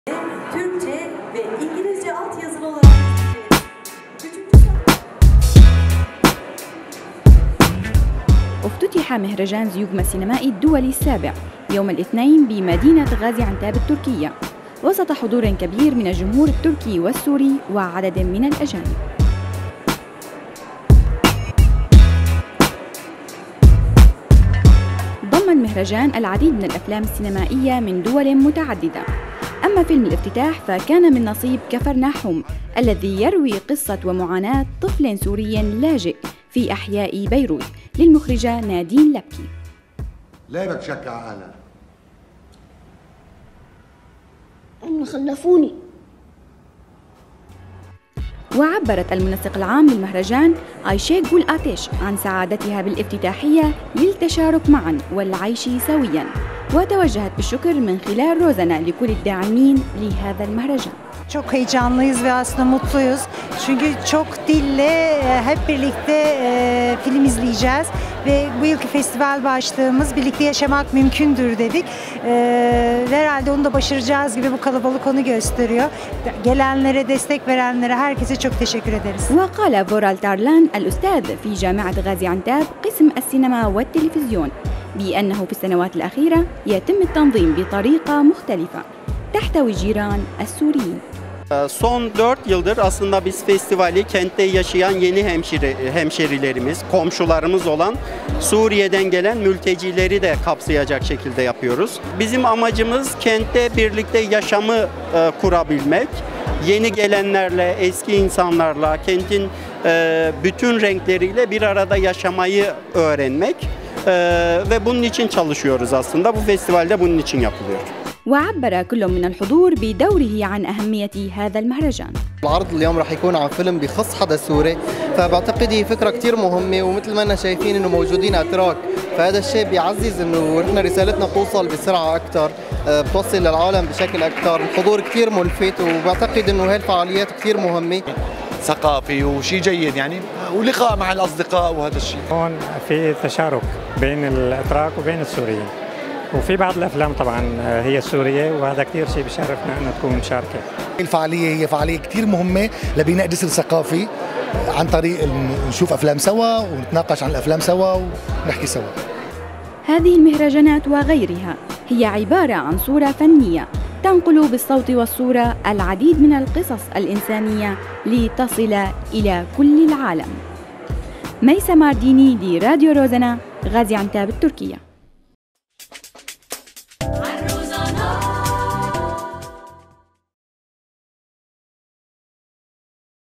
افتتاح مهرجان "زيوغما" السينمائي الدولي في غازي عنتاب التركية (تغطية خاصة)
افتتح مهرجان "Zeugma"زيوغما  السينمائي الدولي السابع في مدينة غازي عنتاب التركية، تحت شعار "العيش معاً ممكن!"، وسط حضور كبير من الجمهور التركي والسوري وعدد كبير من الأجانب.

وضم المهرجان الذي افتتح يوم الاثنين الفائت، عدداً من الأفلام السينمائية المحلية والدولية الحاصلة على جوائز من دول متعددة، والتي يتم عرضها بين  "24 – 28" الشهر الجاري، إضافة إلى الأفلام الوثائقية، والأفلام القصيرة، بتنظيم من  جمعية الثقافة التركية KIRKAYAK .

وبحسب ما ذكرت جمعية "كيركاياك" فإن الأفلام التي تعرض من تركيا تترجم إلى اللغة العربية، وأفلام  من الشرق الأوسط تترجم إلى اللغة التركية ، وأكثر الأفلام المؤهلة من العام الماضي.

وكان فيلم الافتتاح من نصيب الفيلم "كفرناحوم" للمخرجة اللبنانية نادين لبكي، وهو فیلم درامي واقعي یسلط الضوء علی "زین" الطفل السوري اللاجئ البالغ من العمر 12 عاماً، مكتوم القید الذي یتمرد علی الواقع الذي یعیشه،  ویسعی بکل الطرق للحصول علی أبسط حقوقه، لذا یقرر رفع دعوی قضائیة ضد أهله لأنهما أنجباه.